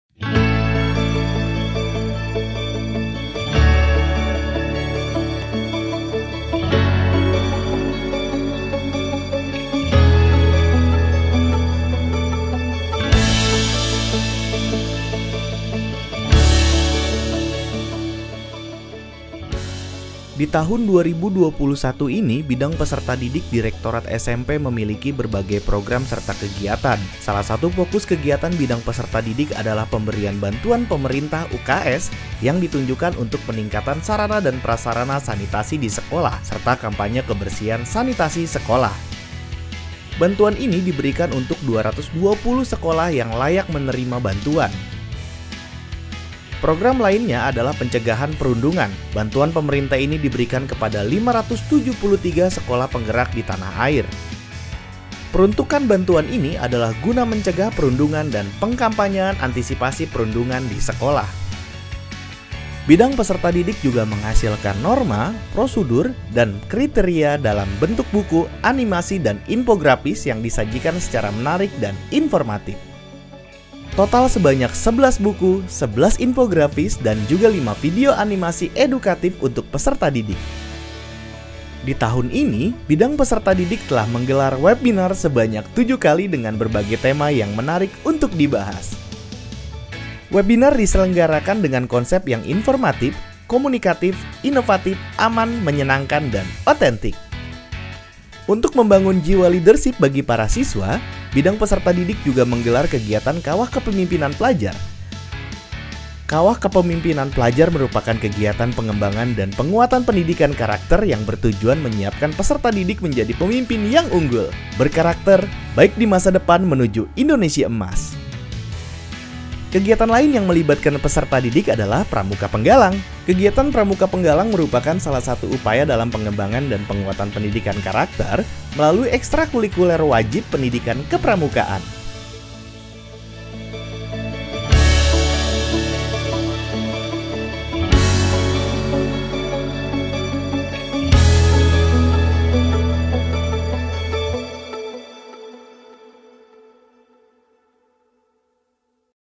Refleksi Bidang Peserta Didik Tahun 2021
Halo, SobatSMP! 👋🏻

Di tahun 2021 ini, Bidang Peserta Didik Direktorat SMP memiliki berbagai program serta kegiatan. Salah satu fokus kegiatan Bidang Peserta Didik adalah pemberian bantuan pemerintah UKS yang ditujukan untuk peningkatan sarana dan prasarana sanitasi di sekolah serta kampanye kebersihan sanitasi sekolah. Bantuan ini diberikan untuk 220 sekolah yang layak menerima bantuan.

Yuk, simak selengkapnya pada video berikut untuk mengetahui kegiatan Bidang Peserta Didik lainnya.